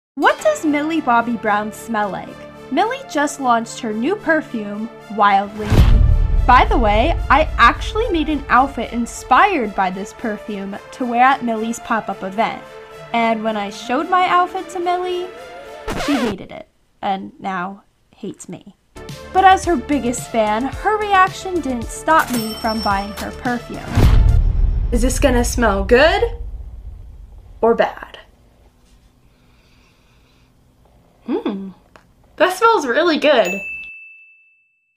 What does Millie Bobby Brown smell like?
What does Millie Bobby Brown smell like? I'm reviewing Millie Bobby Brown's new perfume called Wildly Me from her company Florence by Mills. I actually made an outfit inspired by Millie Bobby Brown's perfume that she hated and now she hates me.